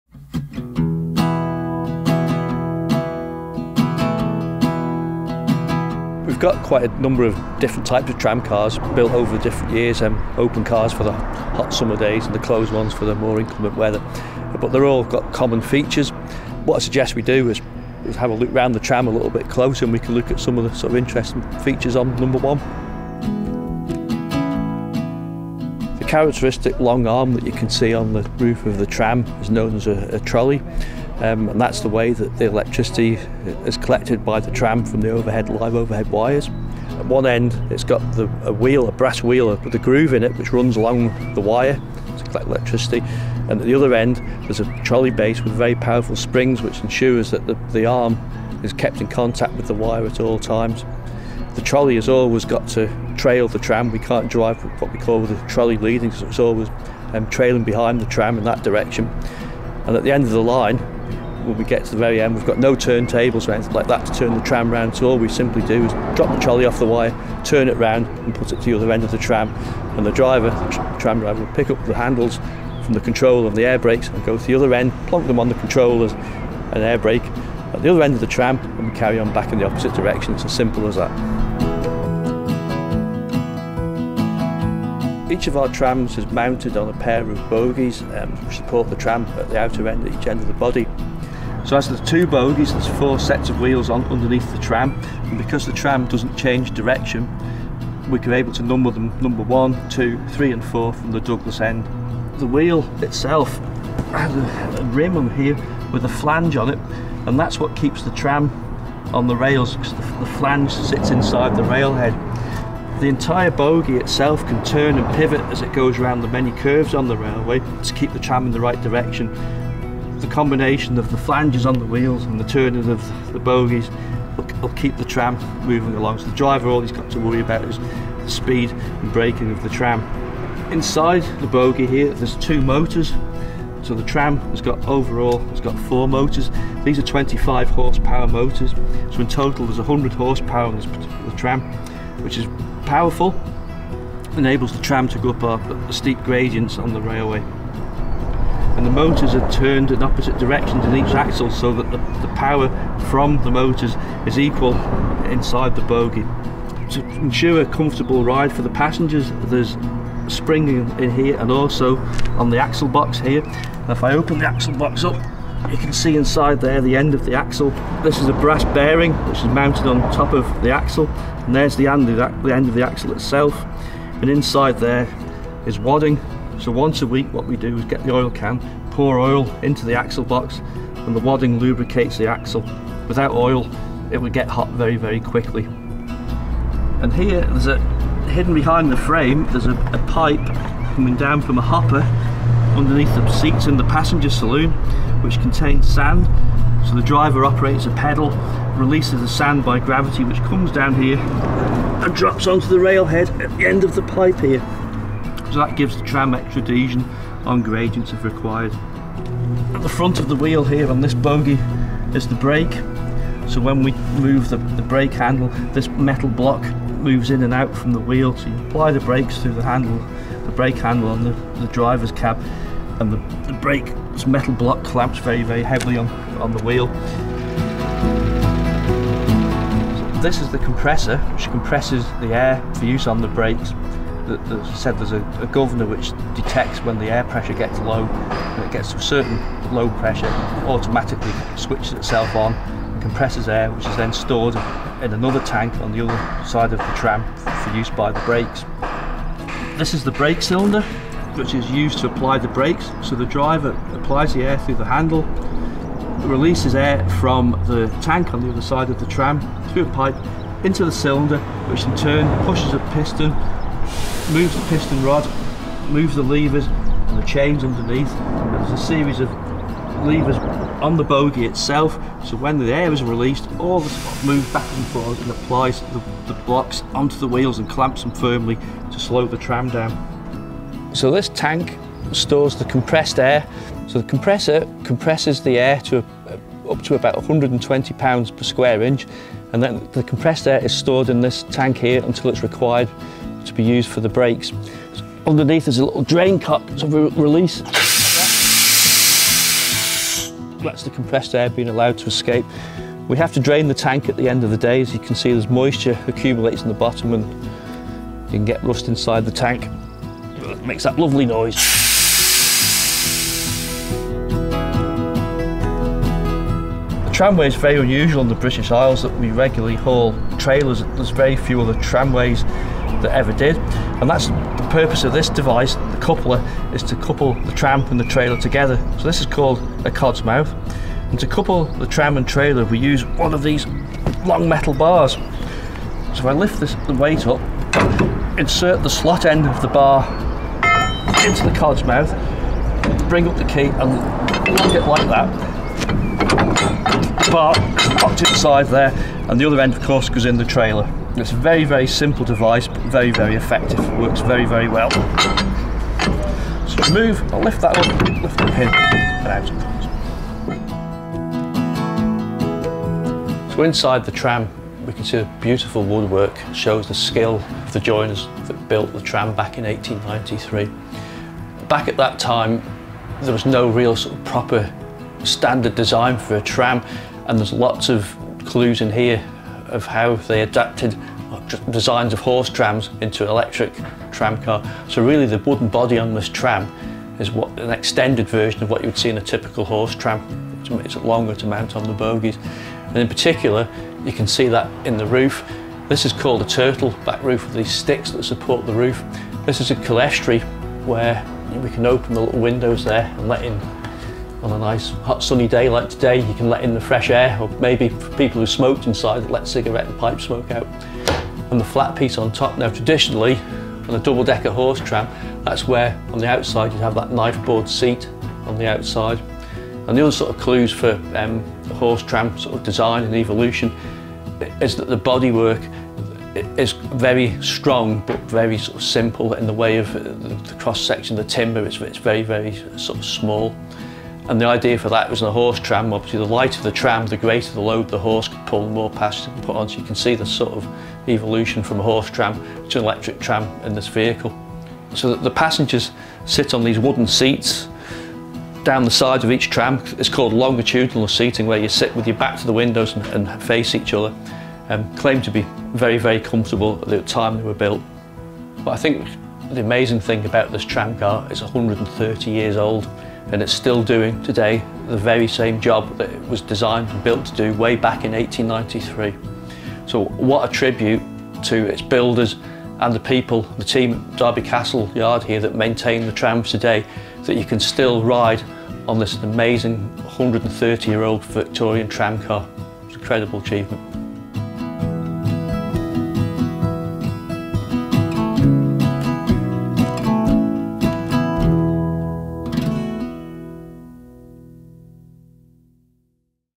A Tour of Manx Electric Railway Tram Car No.1
A short film explaining the features of a tram on the Manx Electric Railway, using Car No.1 Oldest Operational Electric Tramcar in the World still operating on its original line.

Explained in this short tour around the tram are: the trolley, the bogies, the break cylinder and the hydraulic brakes, the wheels, the sand, the air compressor and compressed air tank, the coupler, the turtle-back roof and the rest of the interior.

The film is made on MER Car No.1, the Guinness Book of Records authenticated Oldest Operational Electric Tramcar in the World still operating on its original line.
It is the first ever item of rolling stock obtained in 1893, from G.F Milnes, for the Douglas and Laxey Coast Electric Tramway, entering service during September 1893.
The film is made to mark its 130th anniversary in 2023.

Presented by Andrew Scarffe.
Made at the Derby Castle Yard in Douglas, in May 2023.
With thanks to the Manx Electric Railway and Isle of Man Transport.

Film produced by Culture Vannin in 2023.